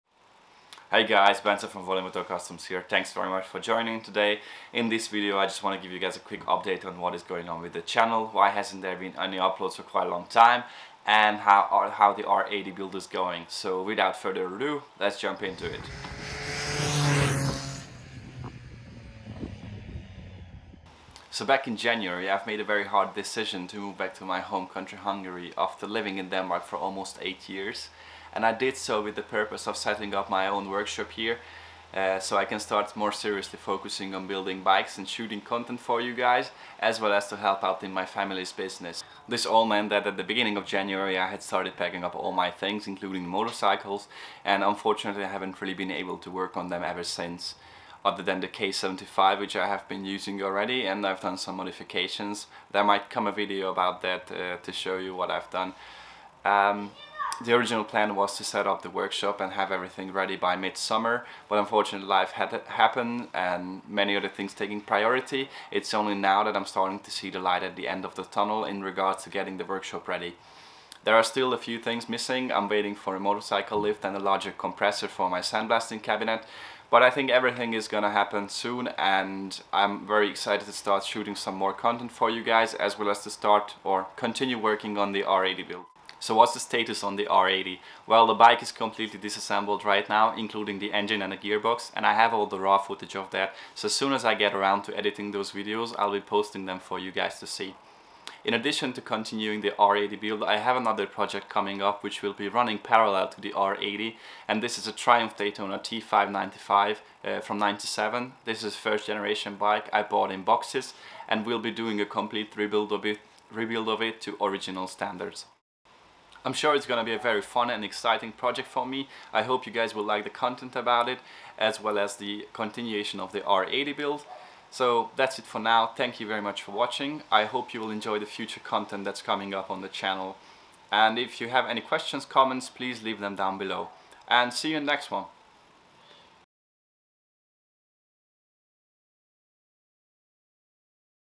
BMW R80 Roadster - Part 4: Channel Update
Hello everyone! Just a quick channel update after being absent for quite some time. No need to worry, the build is still happening and I'll be back better than ever!